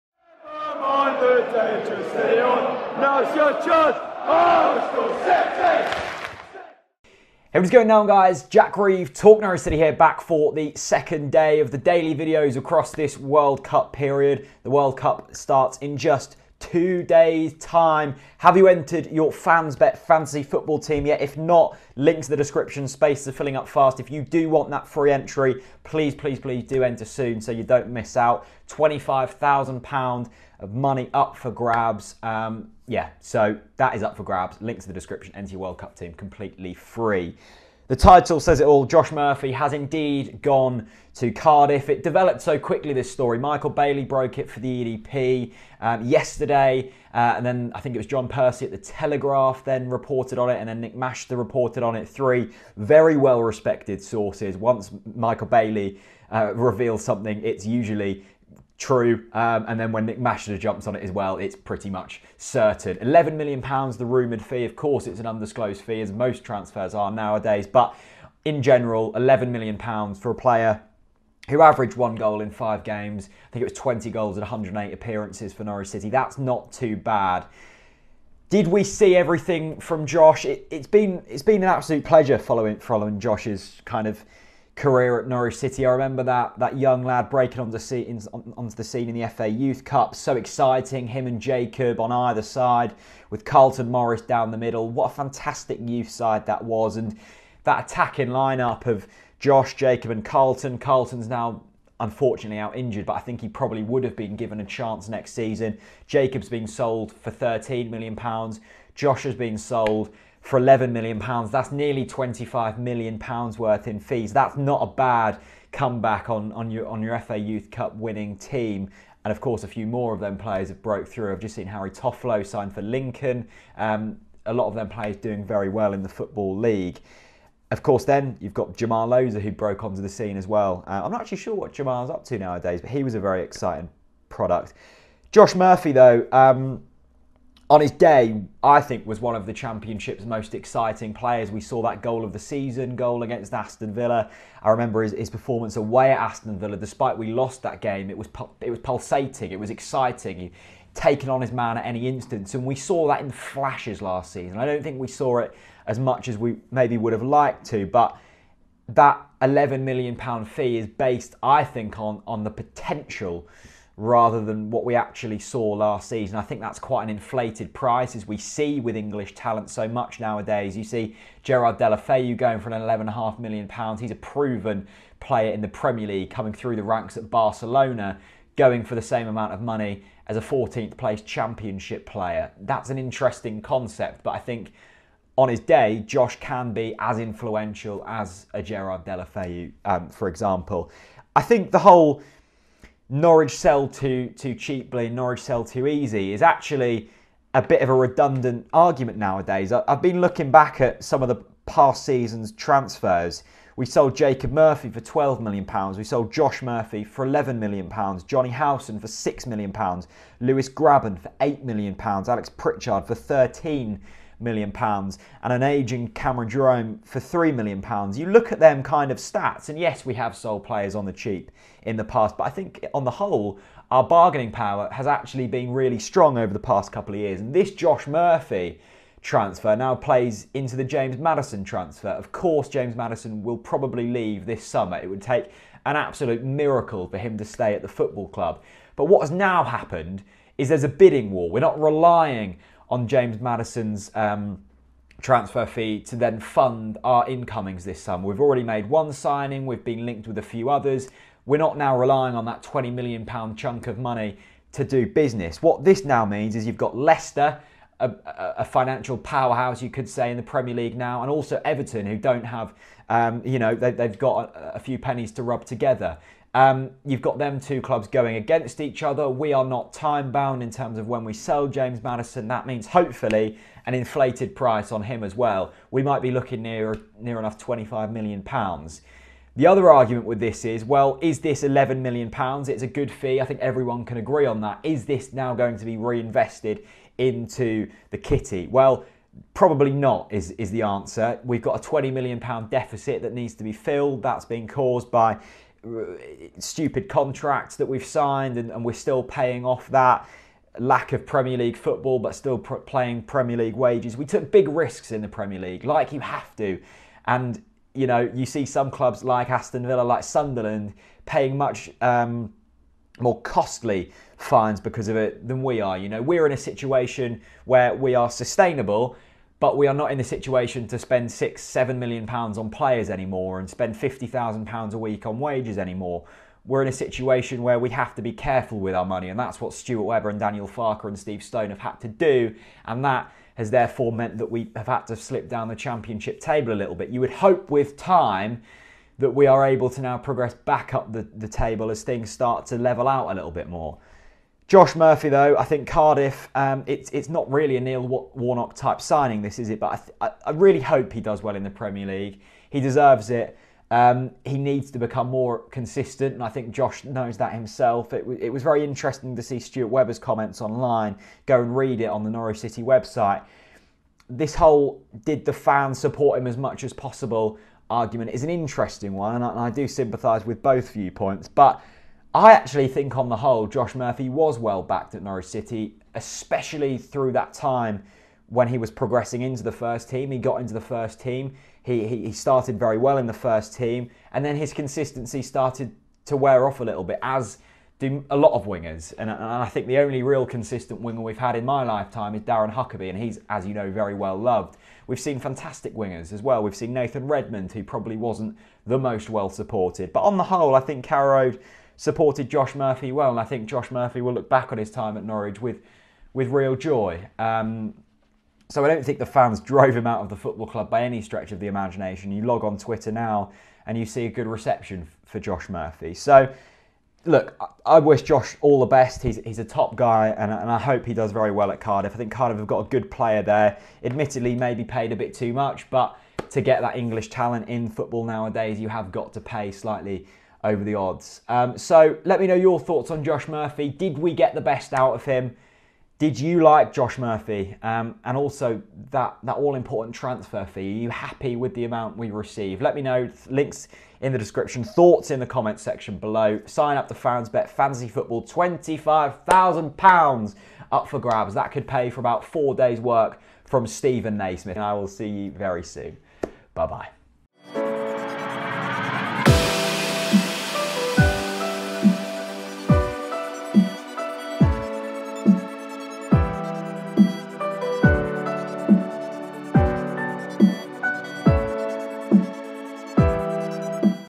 JOSH MURPHY SOLD TO CARDIFF - DID WE SEE THE BEST OF HIM?
So Josh Murphy has gone to Cardiff, did we get to see the best out of him? Jack discusses the situation.